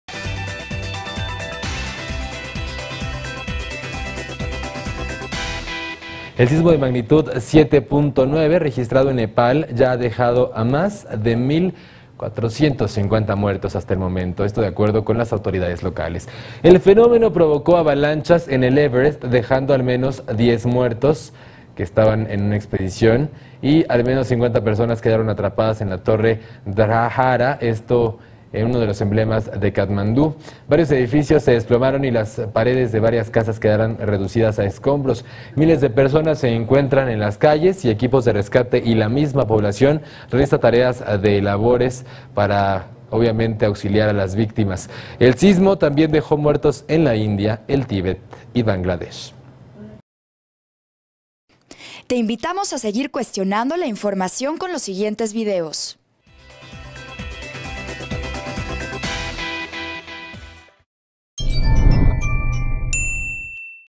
ÚLTIMA HORA: Terremoto en Nepal provoca avalanchas en el Everest
El terremoto en Nepal provocó avalanchas en el Monte Everest.

25 de abril 2015